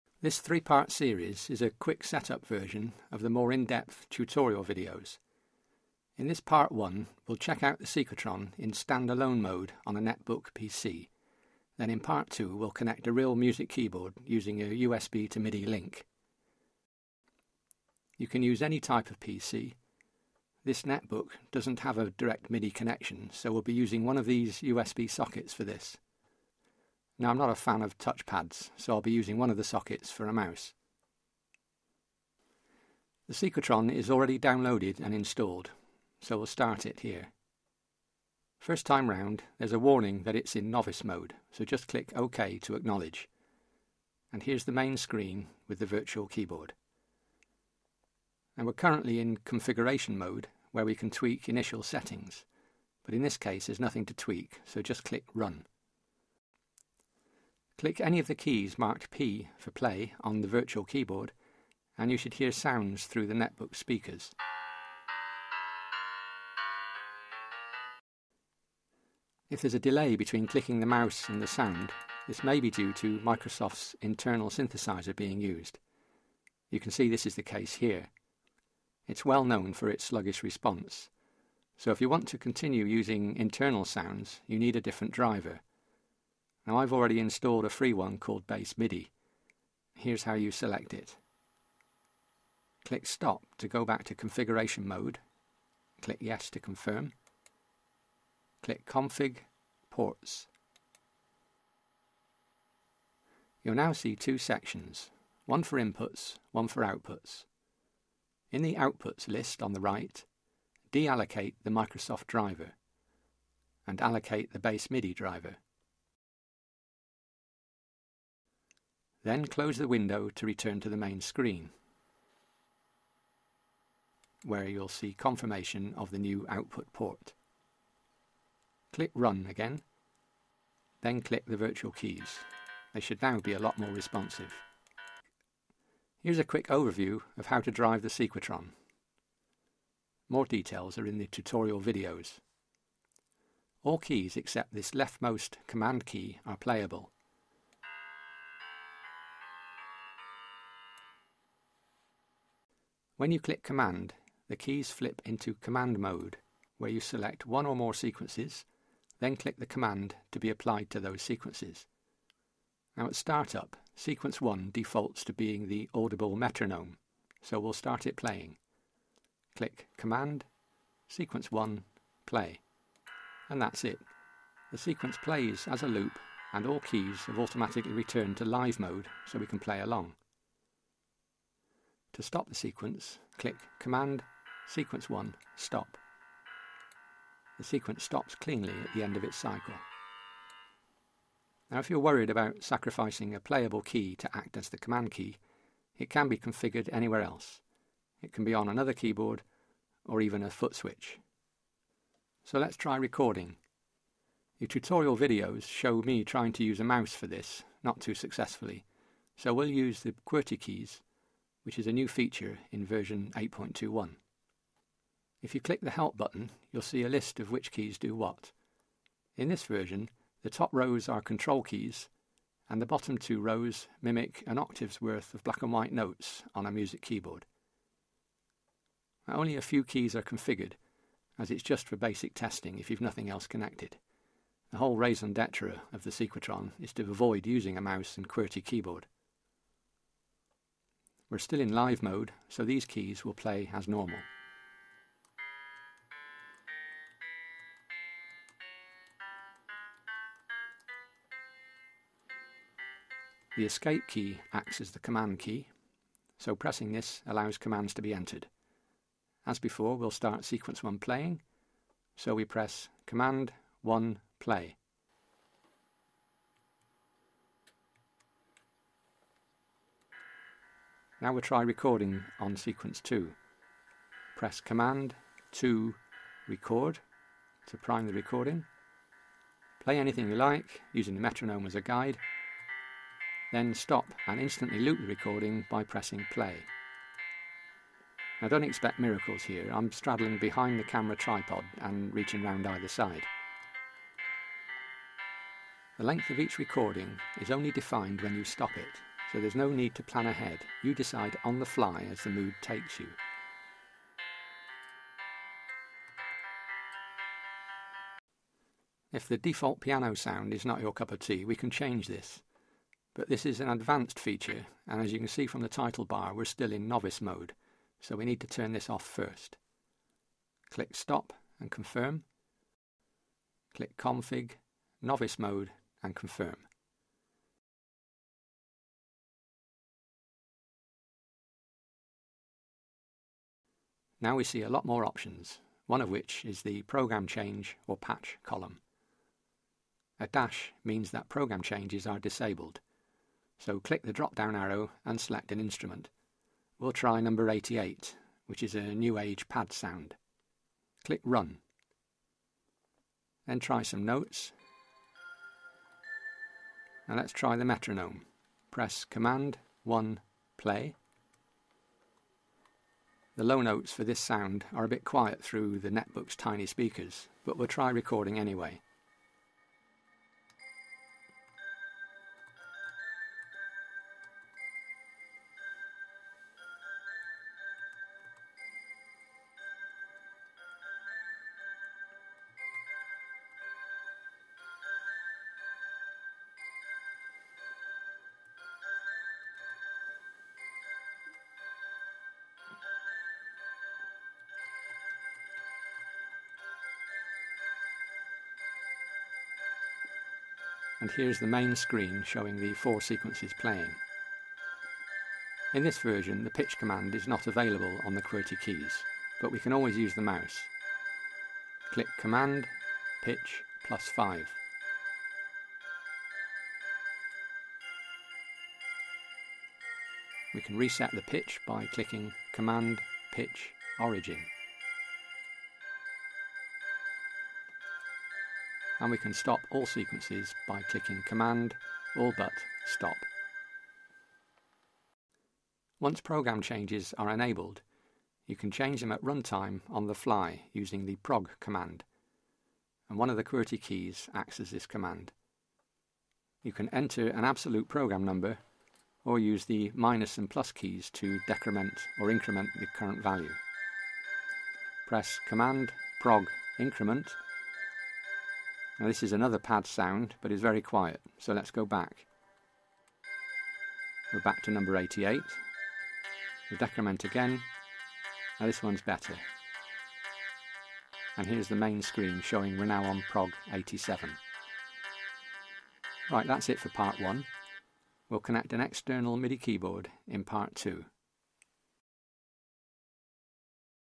Sequetron 8.21 Quick Setup, part 1 - Stand-alone testing
Using the virtual keyboard & qwerty keys on a netbook PC